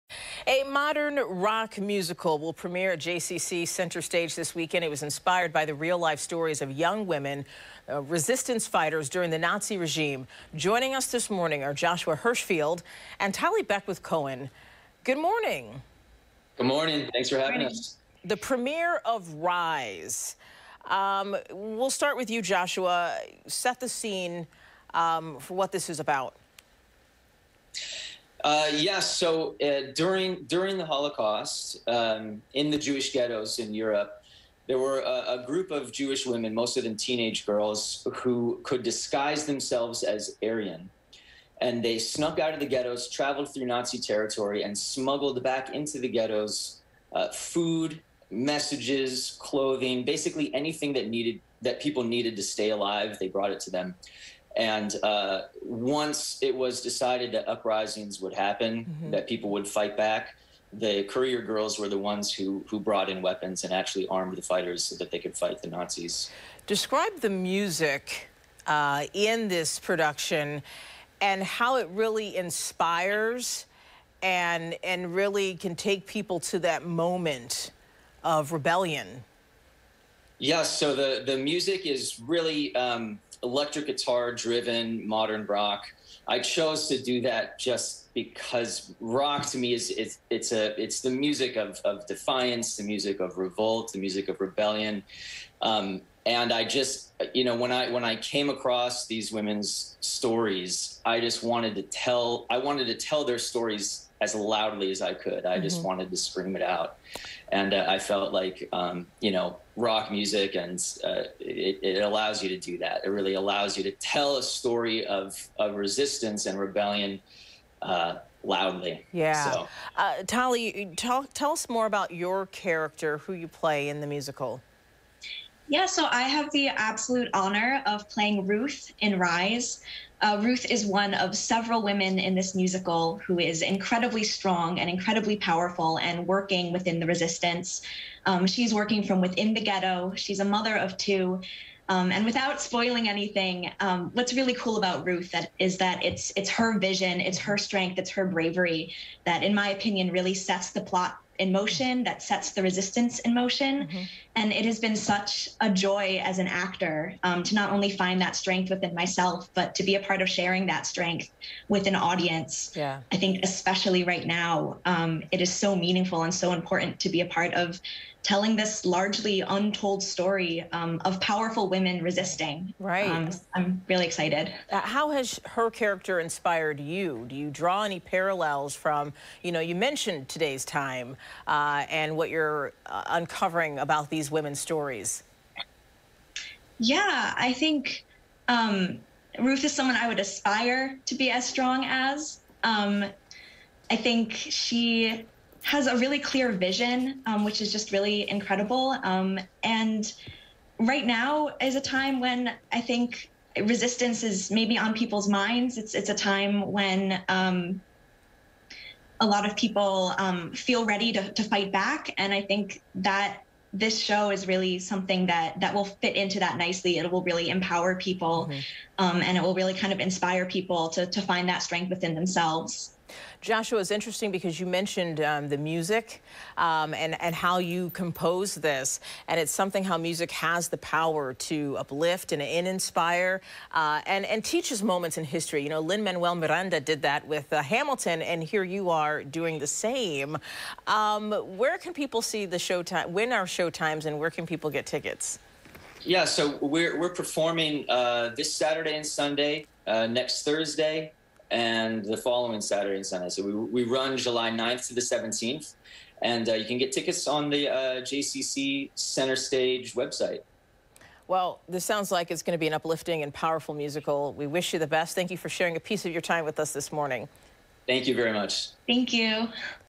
JCC to present new musical "Rise"
The newly produced musical called "Rise" will debut at the Jewish Community Center's (JCC) Hart Theatre from July 9-17.

The musical is a modern rock musical inspired by real life stories of young women resistance fighters during the Nazi regime

Since the characters are mostly in their teens and early 20s, the JCC decided to state the show as part of their summer arts program that uses high school and college musical theatre students from around the region.

Ahead of the premiere, writer and composer Joshua Hershfield and cast member Tali Beckwith-Cohen who plays Ruth, joined Good Day Rochester to discuss how this show came to life and what audiences can expect from its run of shows this month.

For more information and to purchase tickets, visit jccrochester.org.